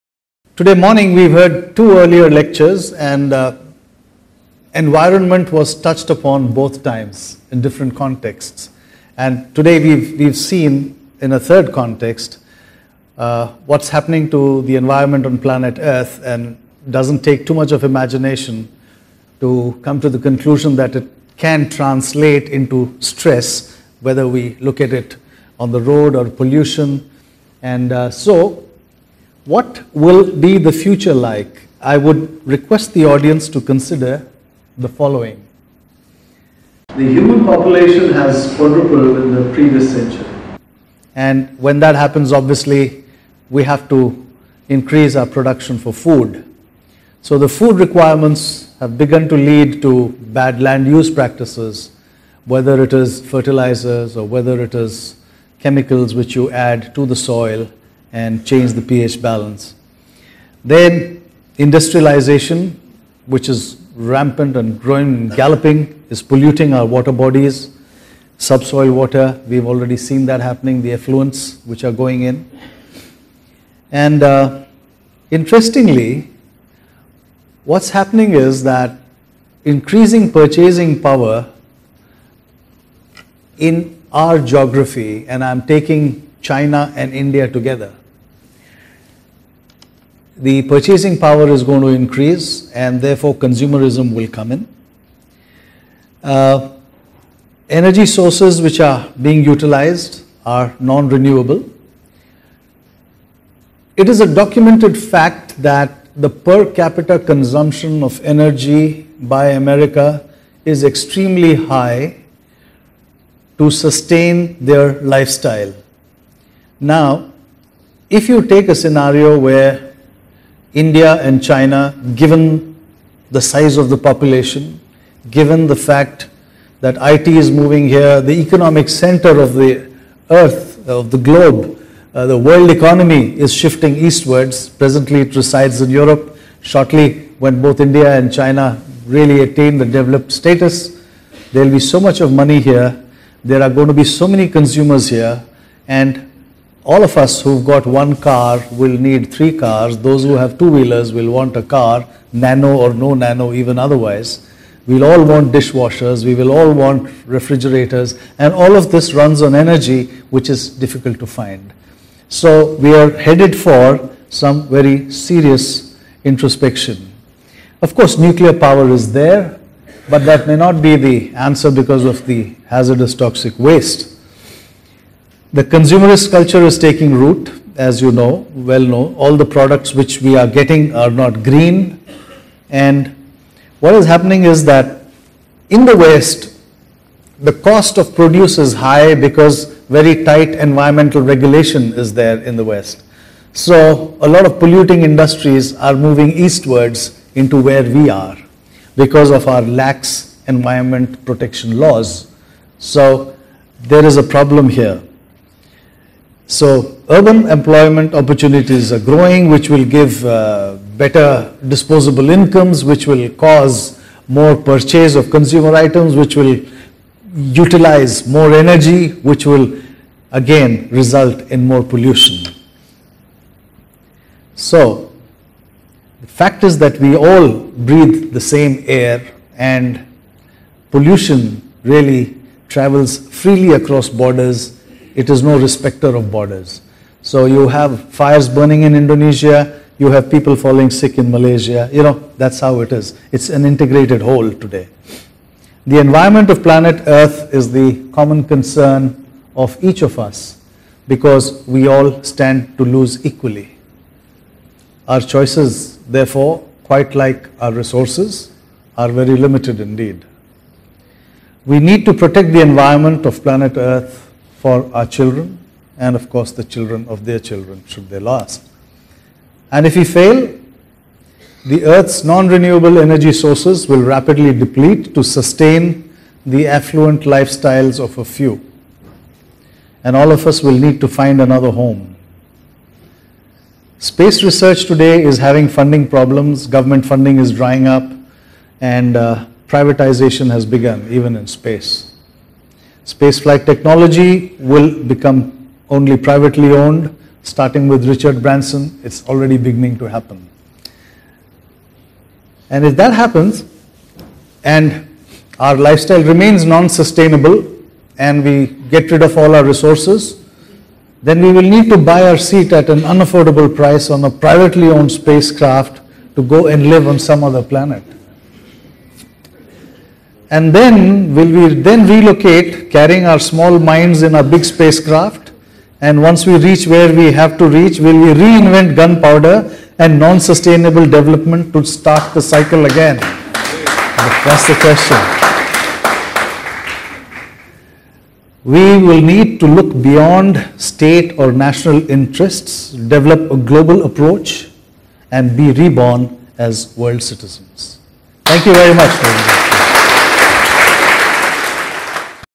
Cosmonaut Rakesh Sharma
Cosmonaut Rakesh Sharma  speaks on "Journey in the outer space" at GAPICON 2008 conference at Goa, India on 3rd February 2008. He strongly feels having a holistic picture of the mother earth from the top, our home "Earth" though teaming with life and beauty is very fragile and it is the ardent duty of each one of us on this Earth to protect the environment and the energy resources of this planet for our Children and the Children of their Children.